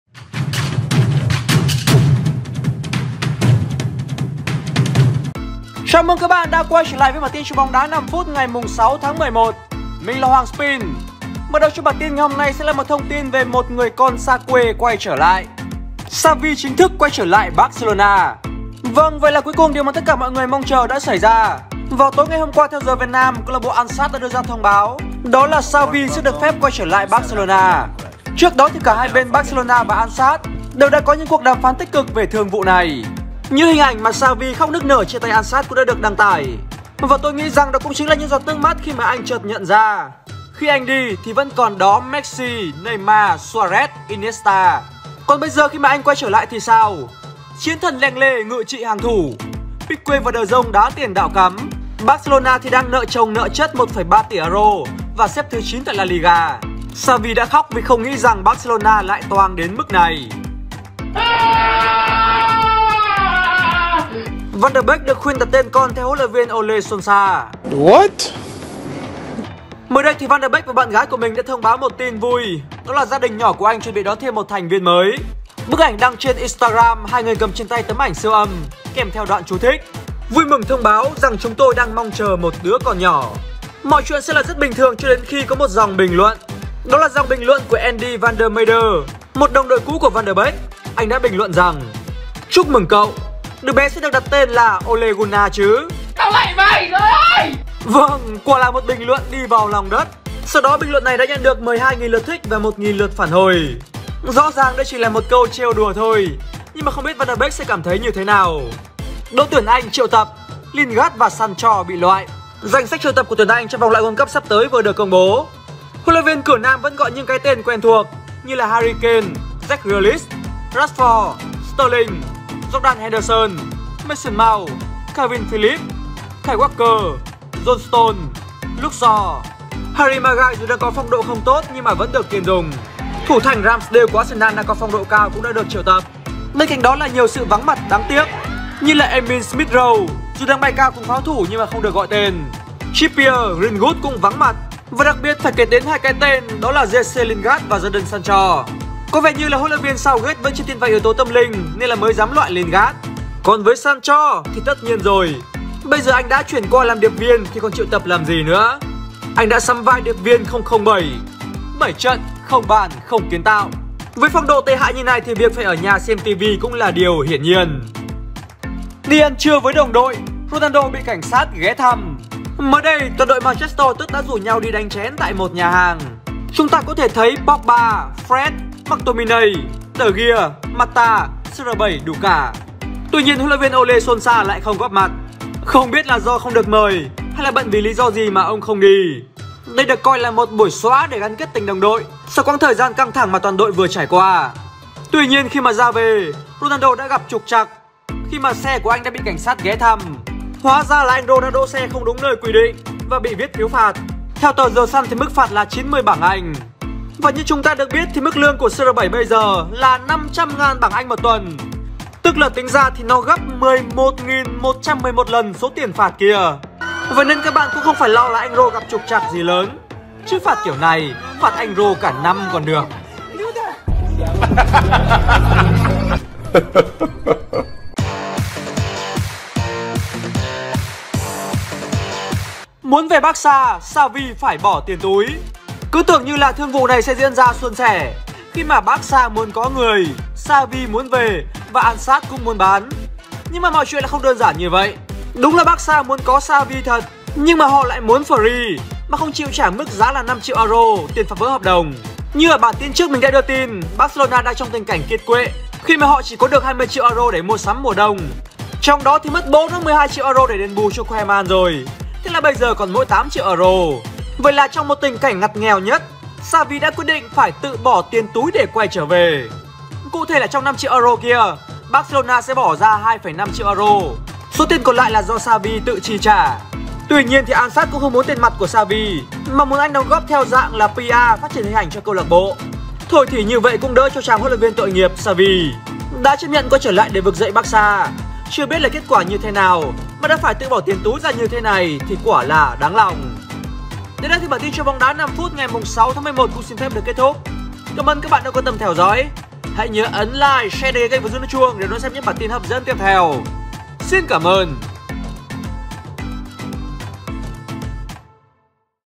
Bản tin Troll Bóng Đá 6/11: Xavi về xây lại căn nhà cũ
+ Xavi chính thức trở lại Barcelona
+ Van de Beek được khuyên đặt tên con theo HLV Solskjaer
+  ĐT Anh triệu tập, Lingard và Sancho bị loại
+  Đi ăn trưa với đồng đội, Ronaldo bị cảnh sát ghé thăm
+ Muốn về Barca, Xavi phải tự bỏ tiền túi